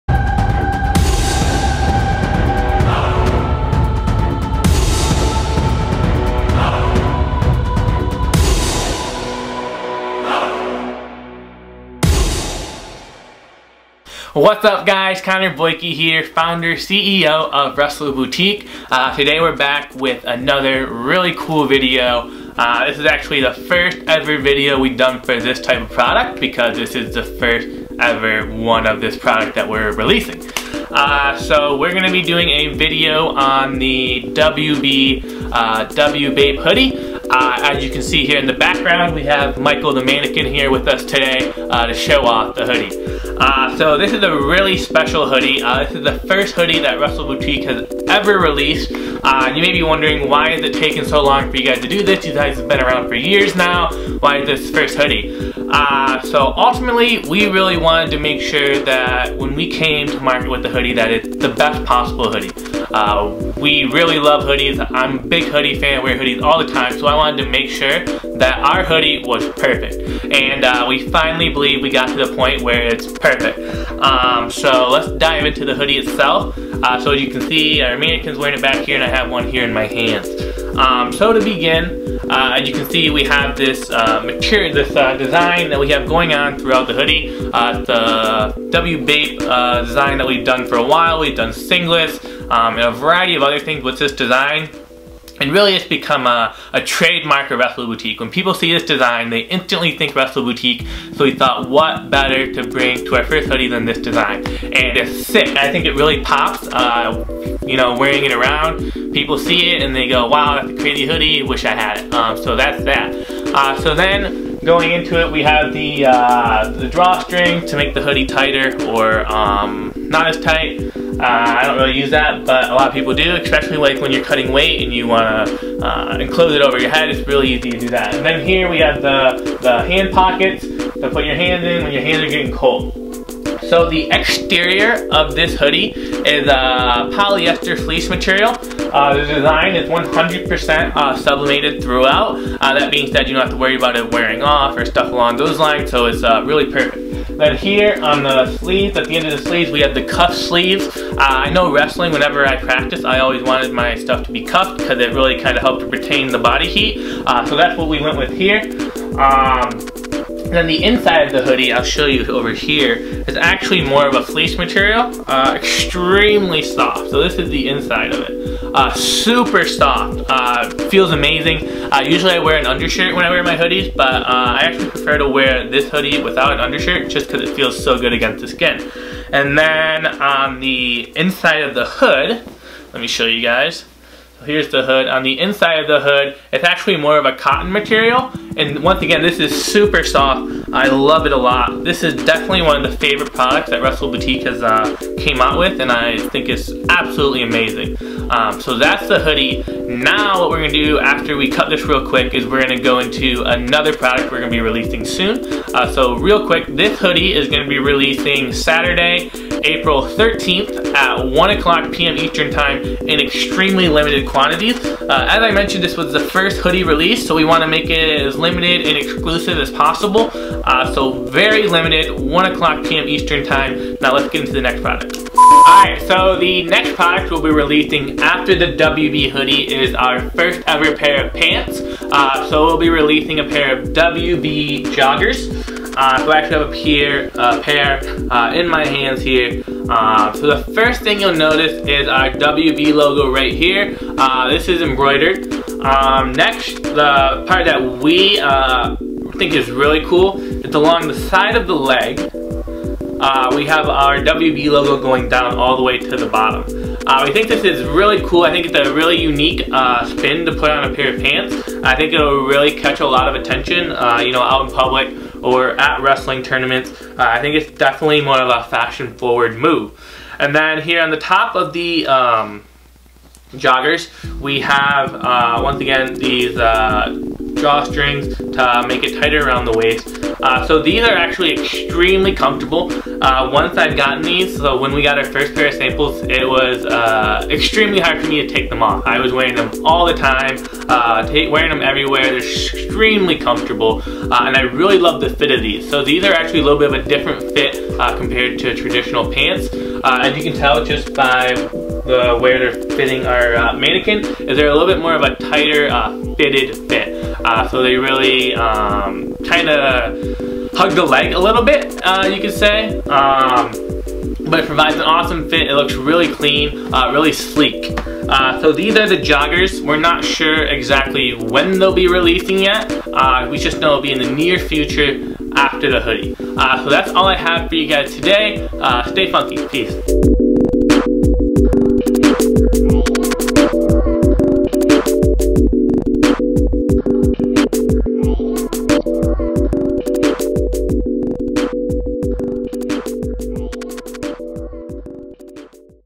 "WBAPE" Hoodie and Joggers Review
The hoodies will be releasing on Saturday, April 13th, at 1:00PM ET The jogger release date will be announced in the near future.